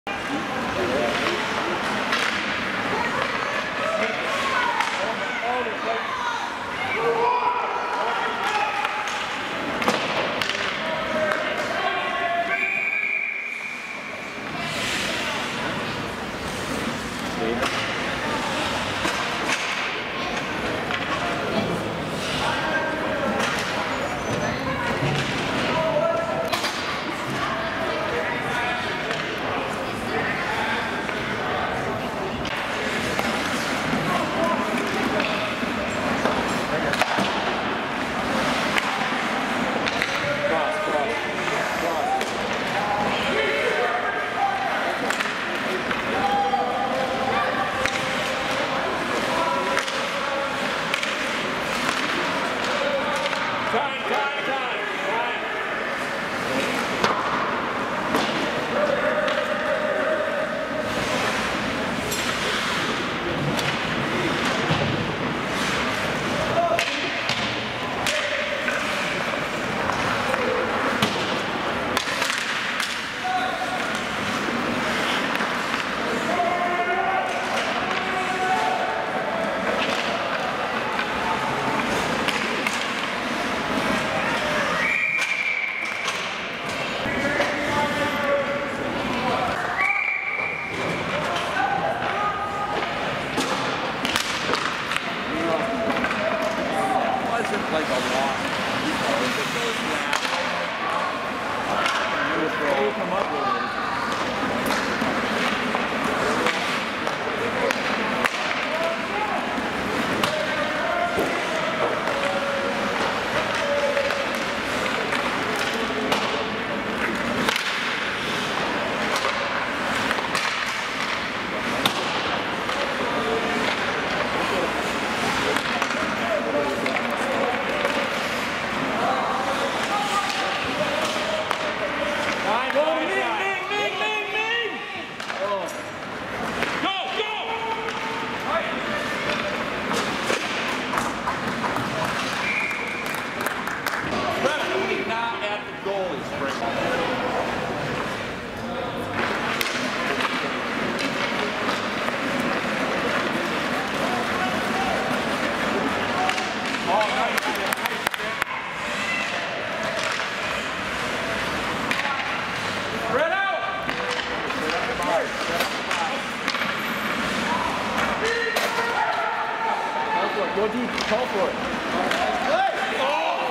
Power Play 1- USA Eagles vs Pittsburgh Arctic Foxes 02/15/21
Powerr Play 1
USA Eagles vs Pittsburgh Arctic Foxes - 02/15/21
Final Score: USA - 5 Pittsburgh - 1
Pittsburgh Presidents Cup - Semi-Final
Southpointe Ice Arena - Pittsburgh, PA